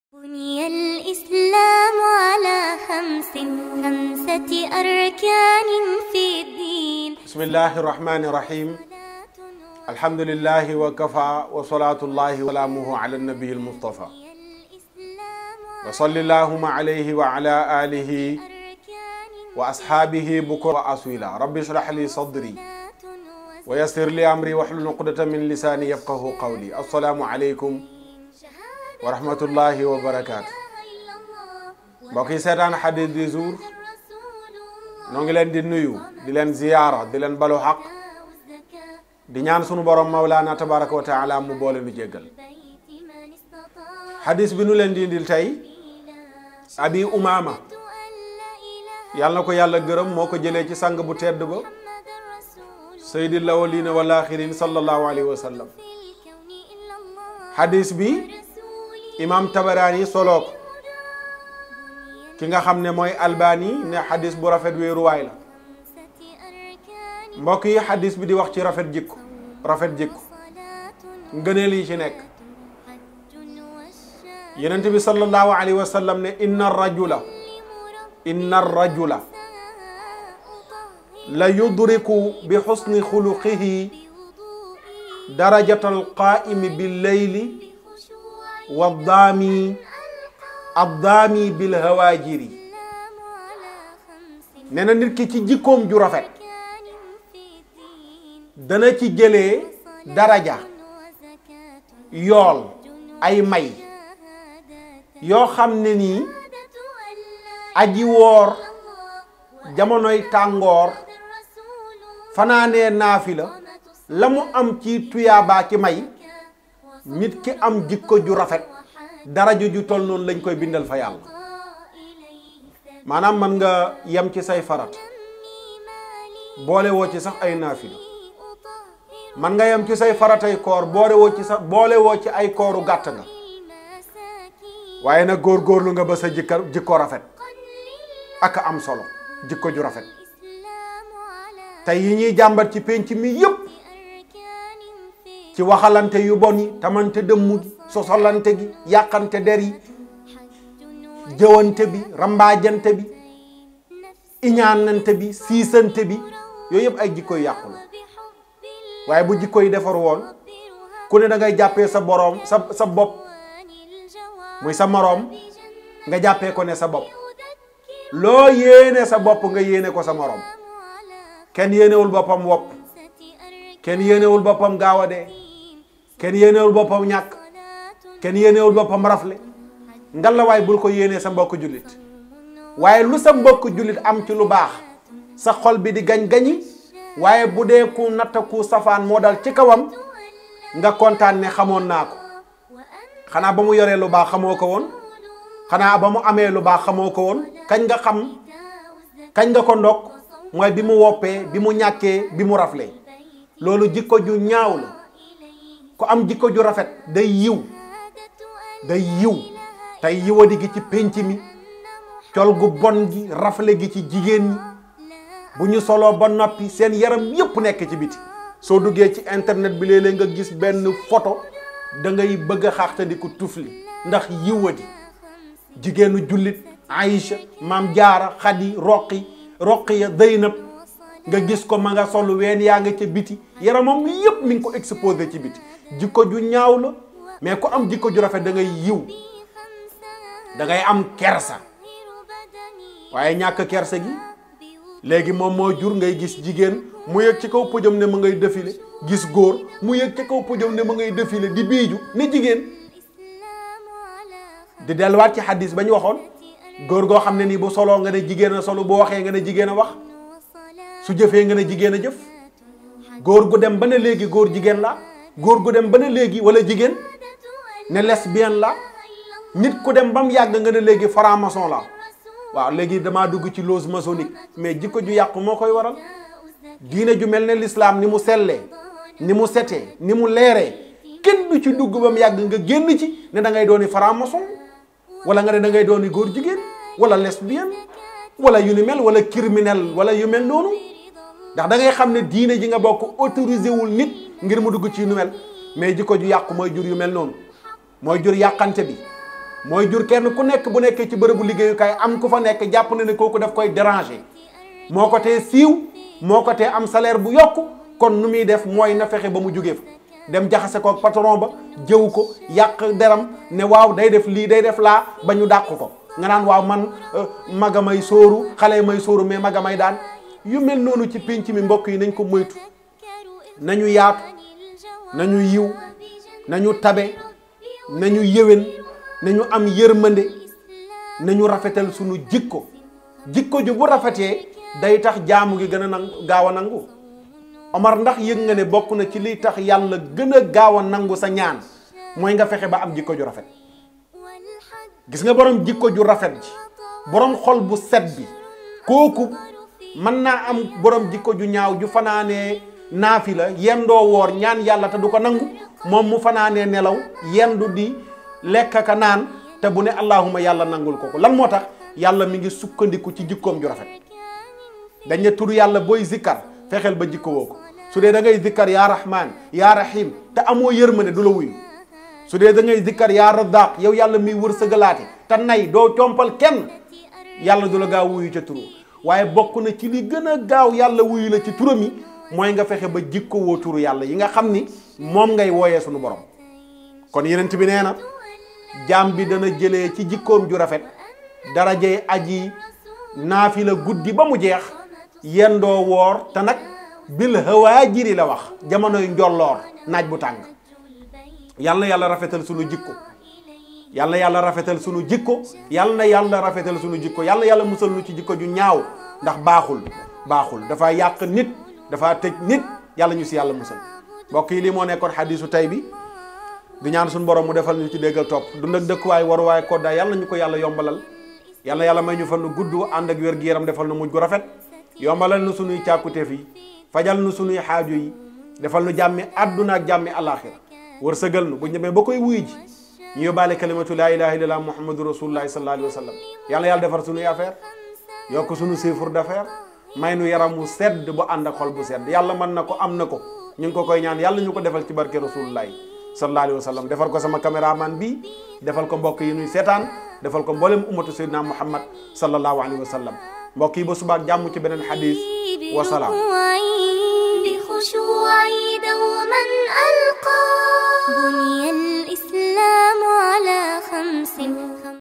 Hadith du Lundi 16 Mars: Certes l'homme atteint par son bon comportement le degré de...
Certes l'homme atteint par son bon comportement le degré de...

D'après Abou Oumama Al Bahili (qu'Allah l'agrée), le Prophète (que la prière d'Allah et Son salut soient sur lui) a dit: « Certes l'homme atteint par son bon comportement le degré de celui qui prie la nuit et a soif au moment le plus chaud de la journée (*) ».
(Rapporté par Tabarani et authentifié par cheikh Albani dans Sahih Targhib n°2644)
(*) Il s'agit de celui qui jeûne.